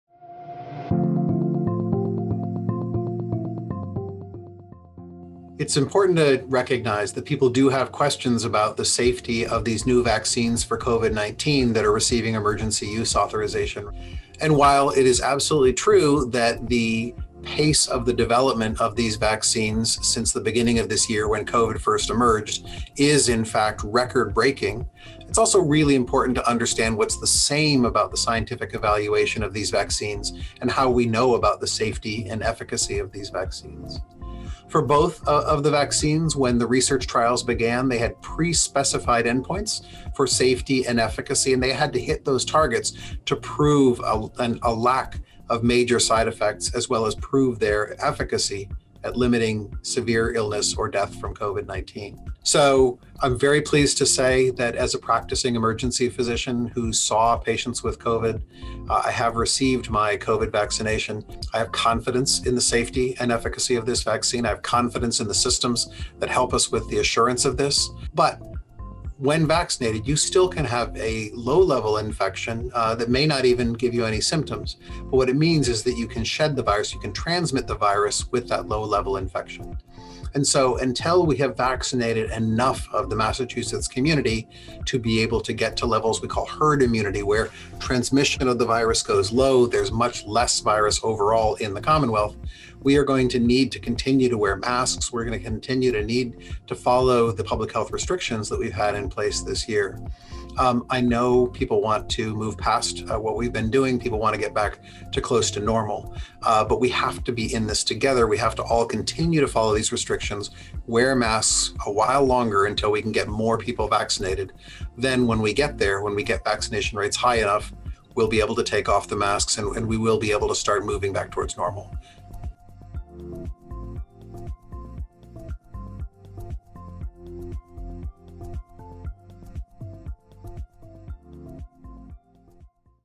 Dr. Paul Biddinger Discusses COVID-19 Vaccine Safety | Mass General Brigham
Hear from Mass General Brigham's Paul Biddinger, MD, Medical Director, Emergency Preparedness, about the safety of the COVID-19 vaccination.